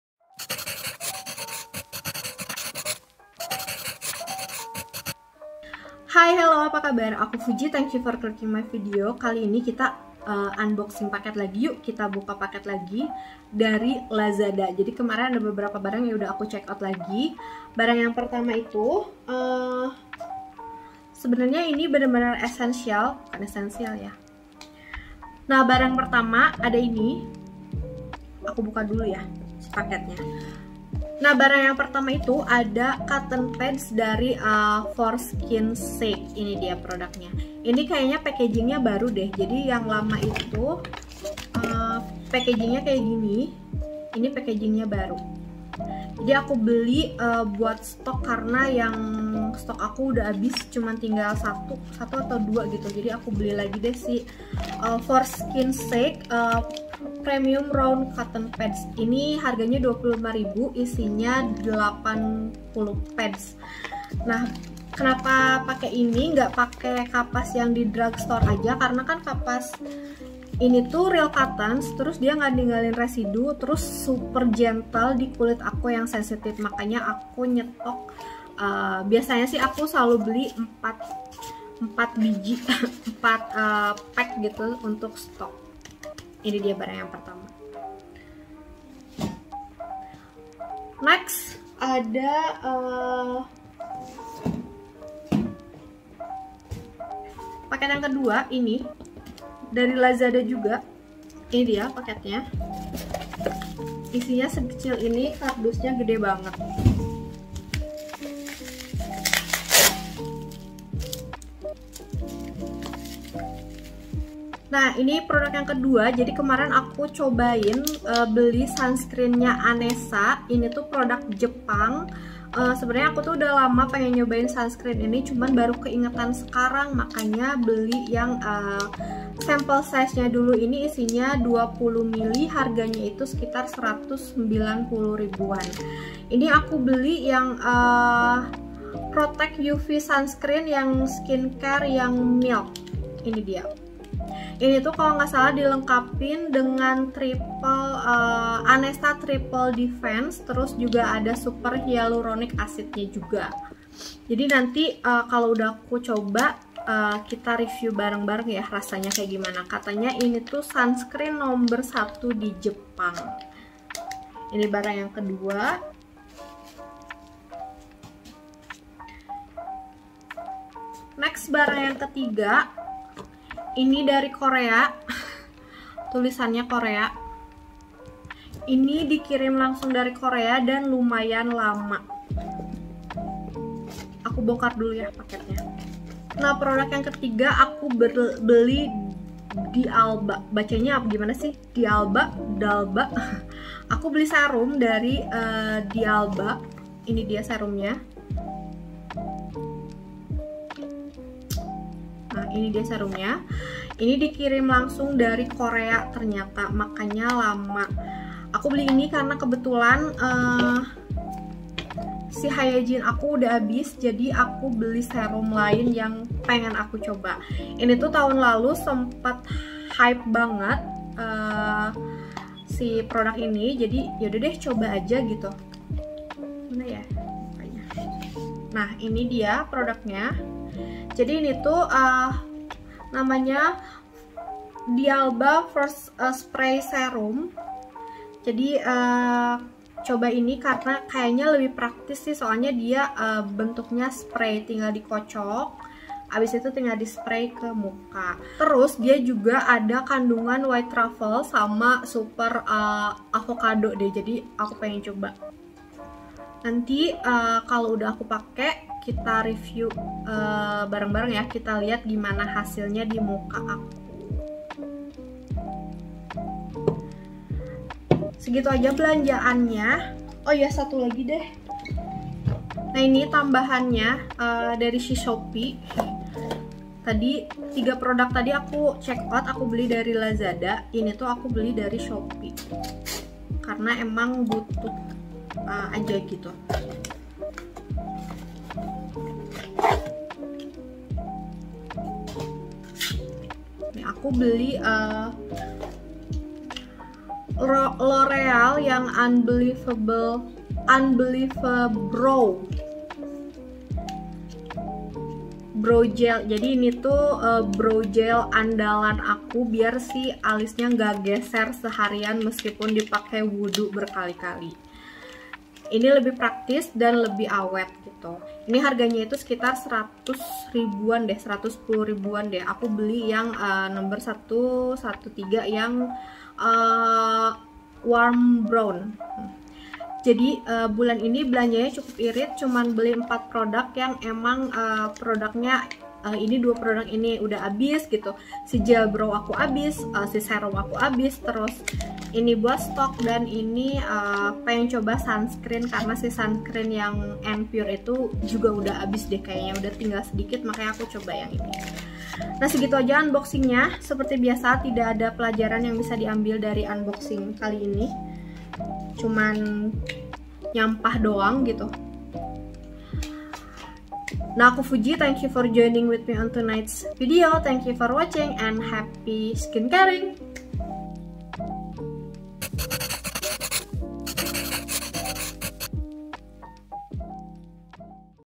Unboxing | Belanjaan dari
Hello, Friends !

Aku Fuji and Thank you guys so much for joining this time and I will see you soon again 🤍🤍🤍

Unboxing lagi yuk..
Kali ini belanjaan essential yg bakal sehari-hari aku pake aja sih.

Product mentioned :
- Premium Round Cotton Pads by For Skins Sake 80pads
- Anessa Perfect UV Sunscreen Skin Care Milk Spf 50+ PA 20ml

*all comments are belong to my own 🙃
*I hope you guys found this video helpful
*my skin type is dehydrated and sensitive skin. You may have a diff experience with the product I review.